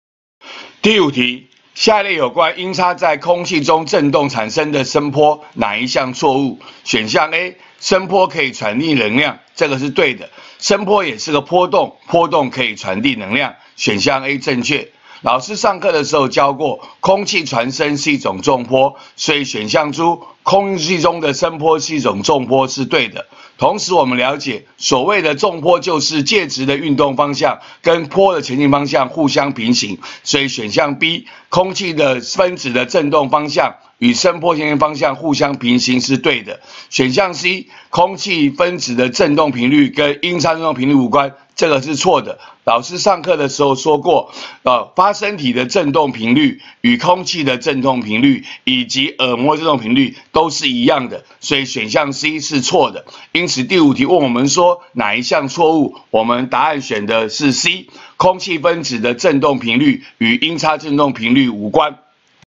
106學年上8年級理化二段第05題
問有關音叉在空氣中振動產生的聲波，敘述何者錯誤。